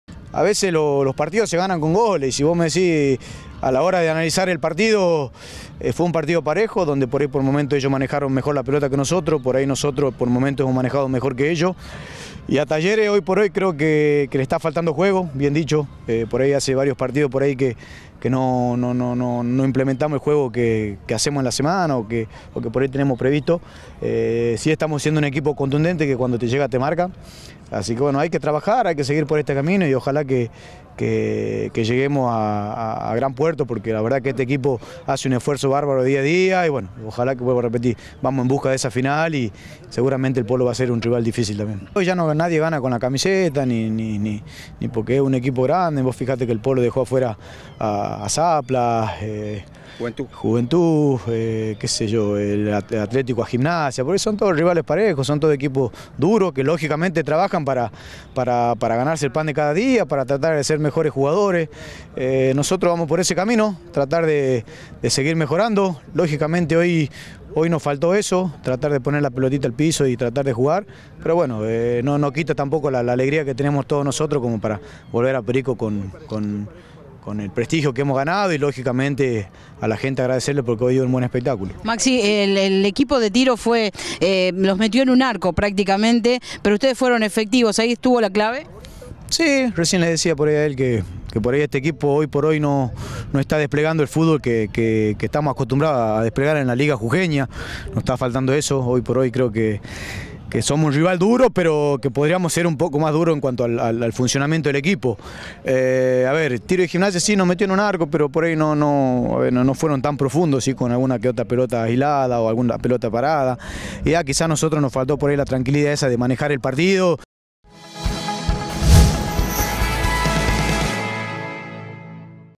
Copa Jujuy Talleres se convirtió en el último semifinalista
En San Pedro, venció a Sociedad Tiro y Gimnasia de esa ciudad por 1 a 0.